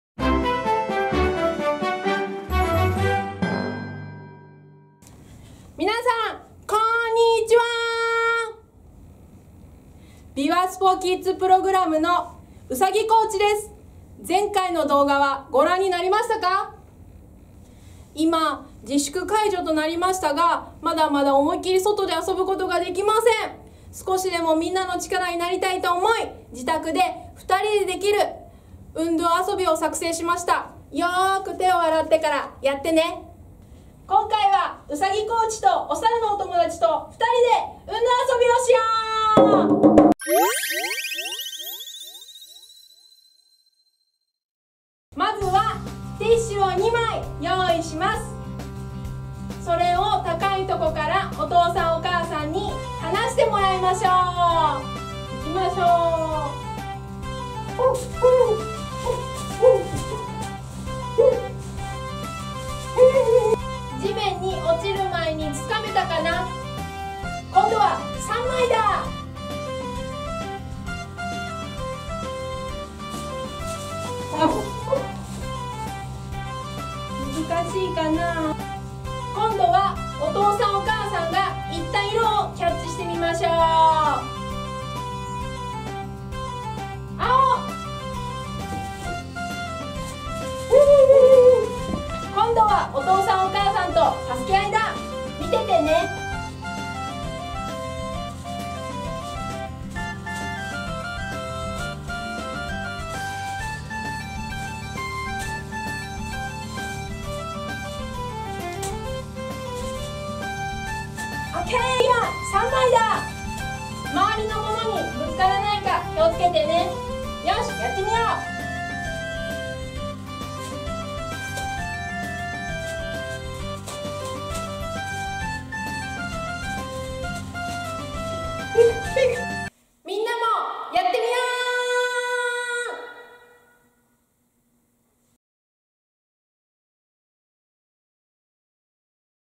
第3回びわスポキッズプログラム運動遊び～2人でできる運動遊び②～
「びわスポキッズプログラム」は、2009年7月に開始されたびわこ成蹊スポーツ大学と自治体やスポーツ協会などのスポーツ団体地域の企業等とともに滋賀県のこどもたちの体力向上・健康増進を推進する産学連携事業です。 本プログラムから、まだまだコロナウイルスに負けない！子どもたちに楽しんでもらいたい！みなさんの力になりたい！そんな気持ちで動画を作成しました。ぜひやってみてくだい。